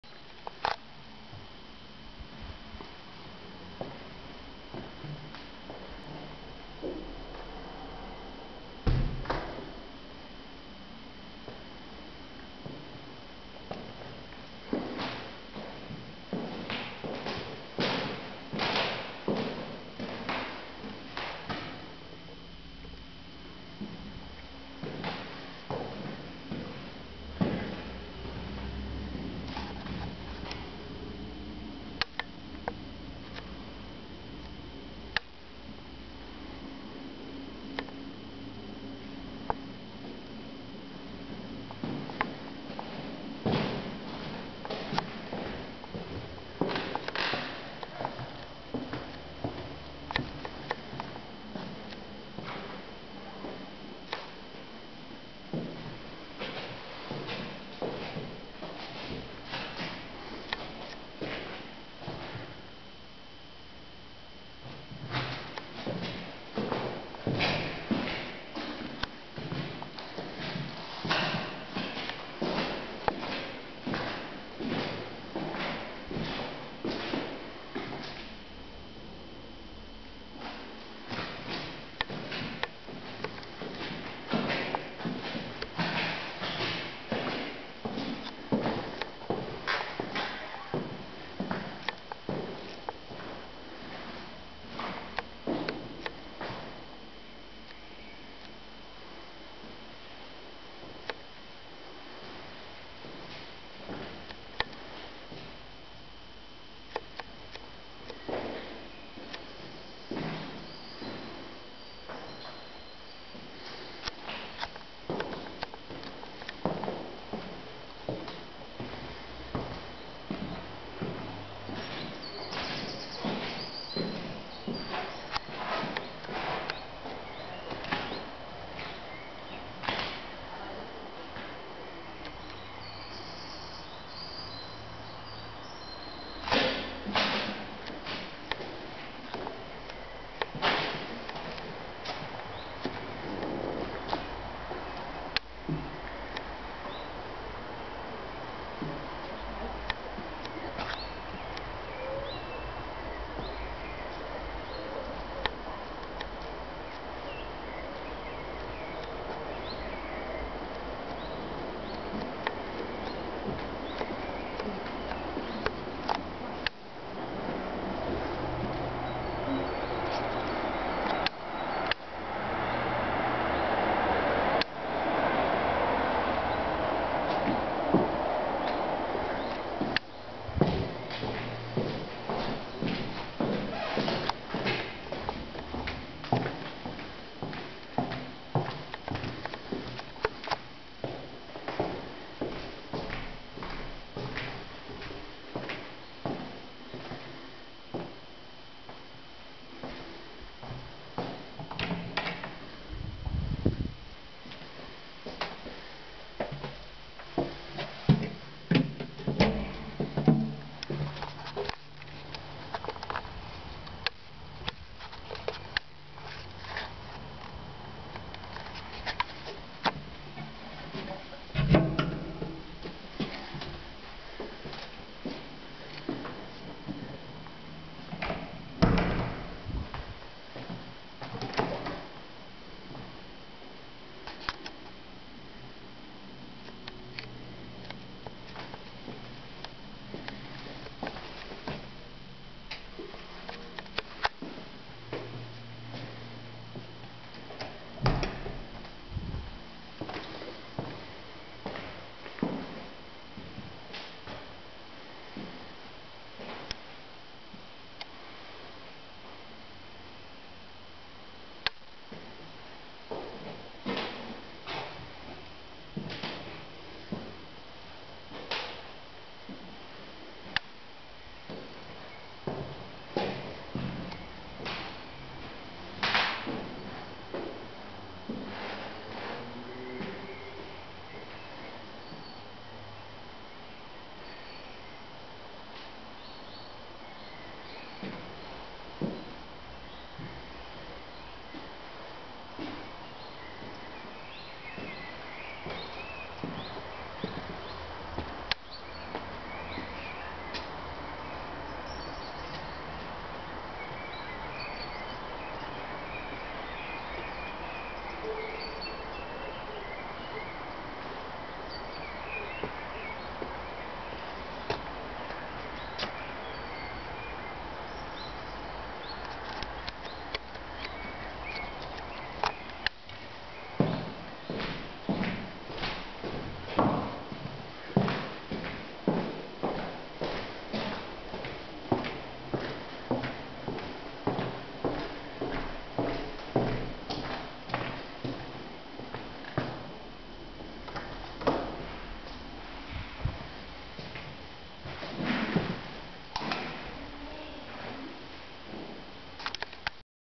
Erster Rundgang nach Übernahme der Wohnung Schweizer Strasse 116, 1. Etage
27. April 2012